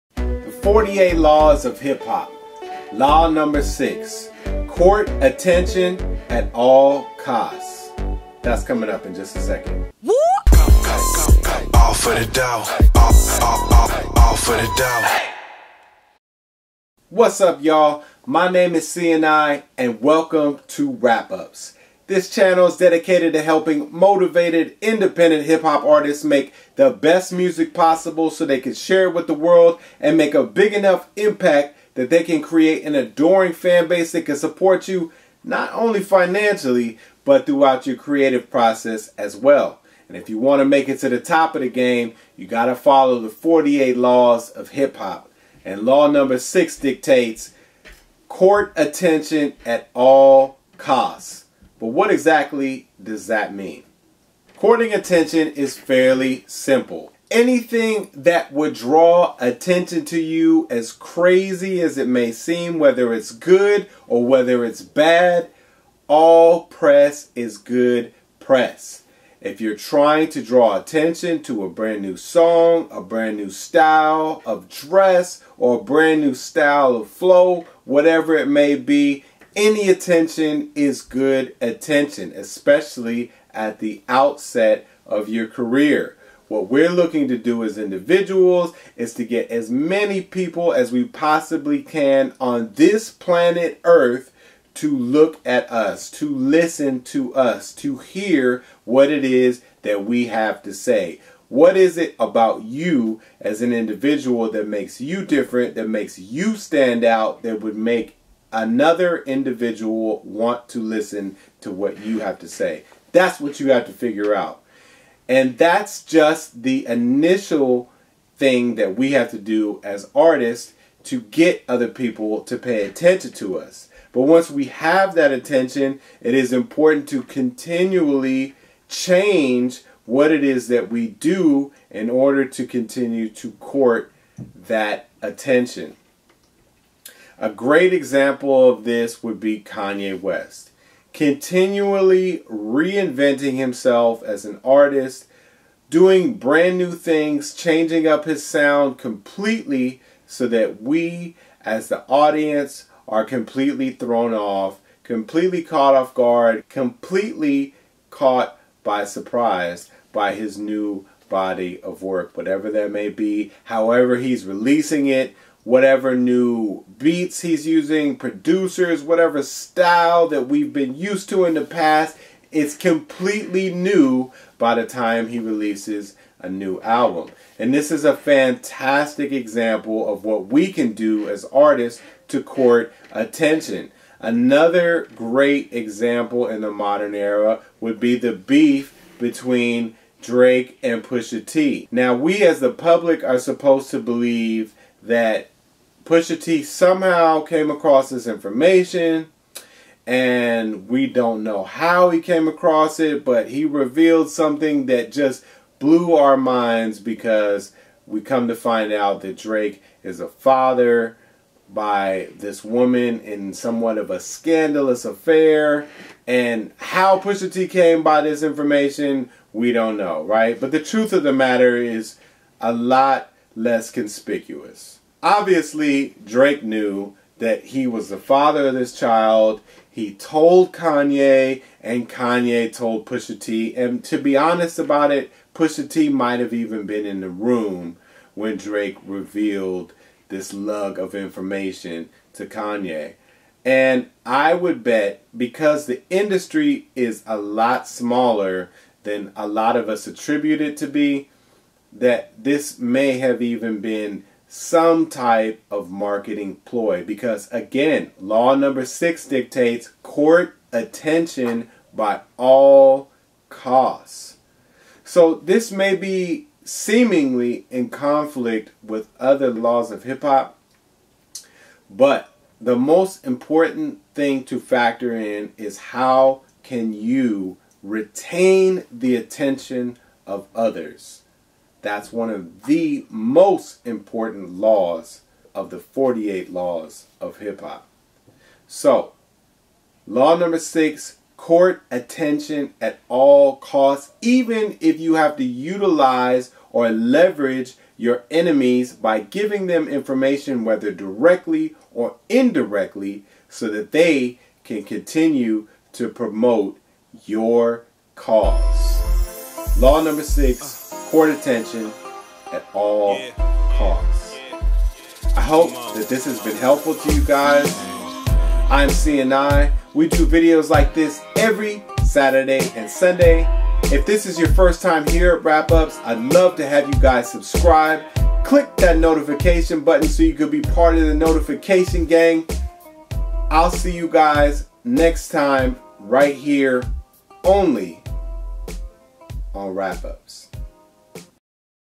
48 laws of hip hop Law #6 Court attention at all costs
The 48 Laws of Power can be applied to all aspects of life. Why not apply them to hip hop as well?. In this episode we'll explore how the sixth law can be applied to the world of hip hop. Law #6 Court attention at all costs !

Ableton Live DAW

1.) Ableton Live 10 Suite Multitrack Recording Software -$799.00